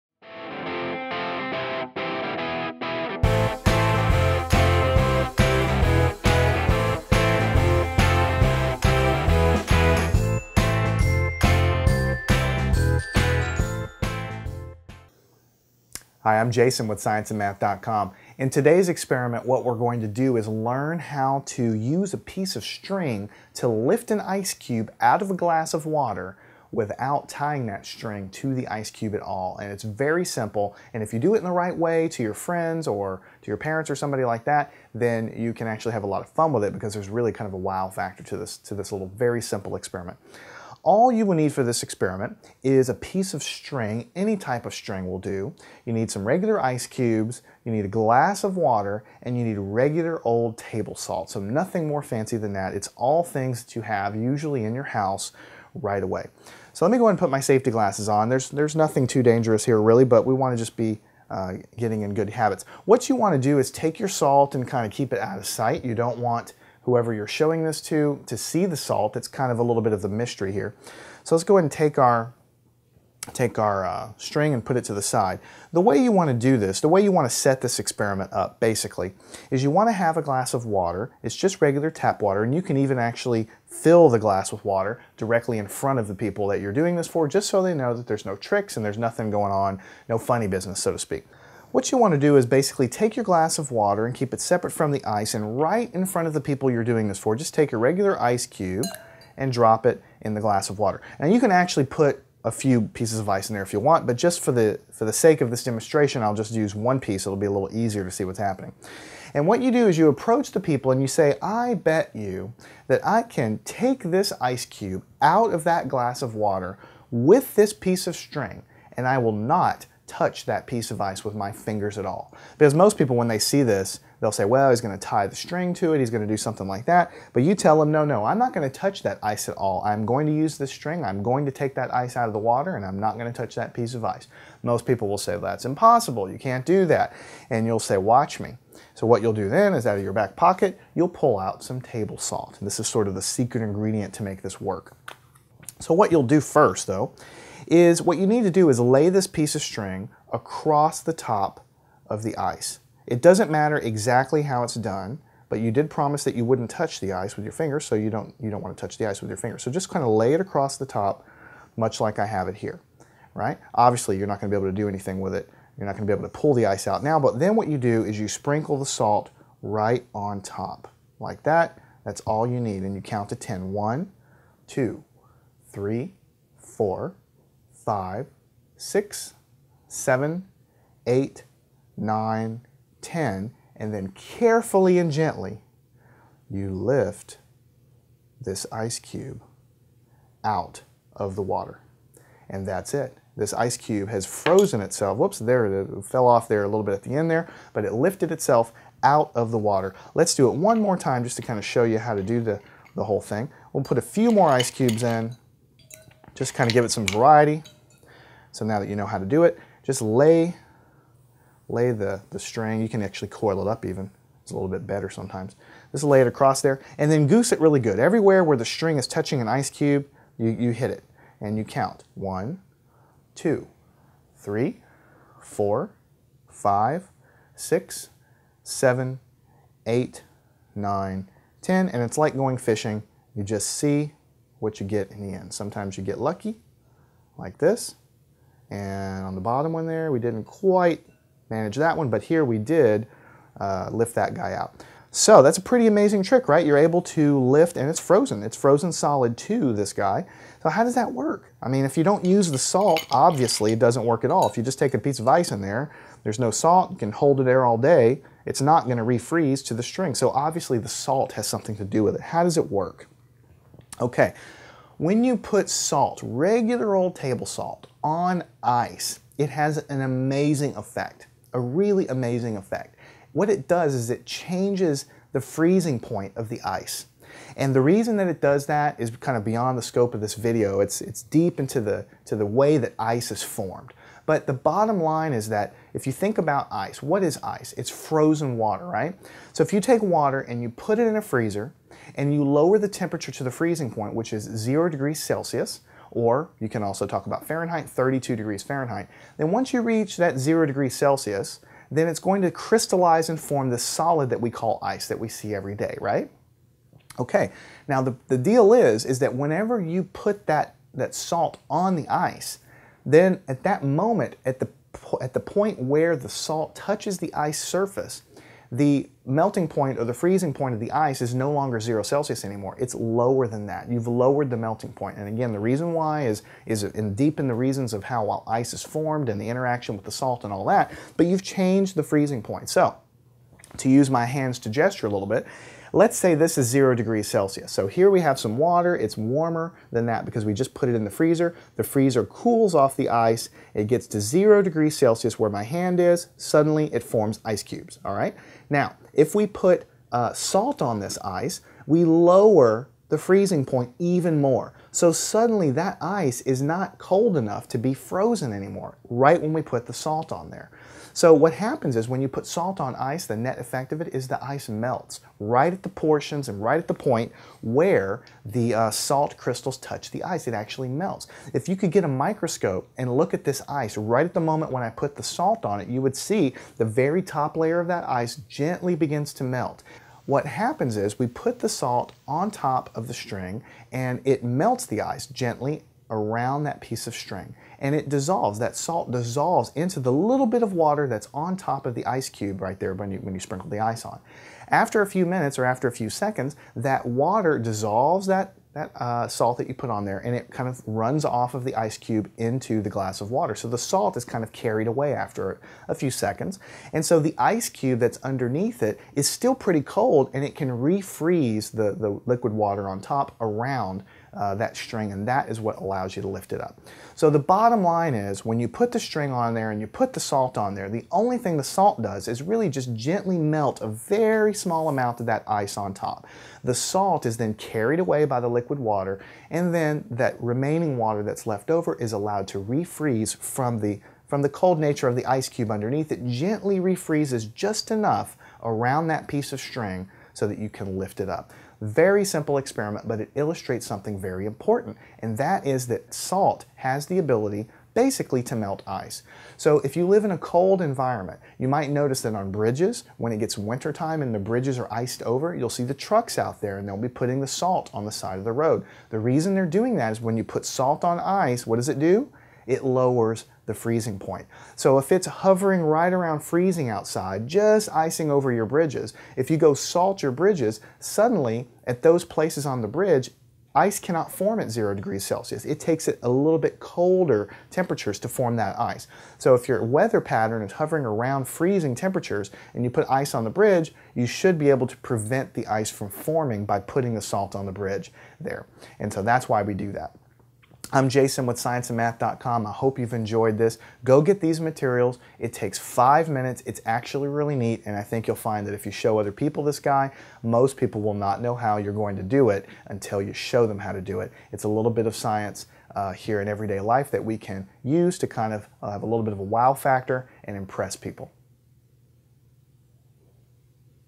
Lift an Ice Cube with String! - Science Experiment - Melting Point and Freezing Point of Water
Learn how to lift an ice cube from a glass of water using only a piece of string!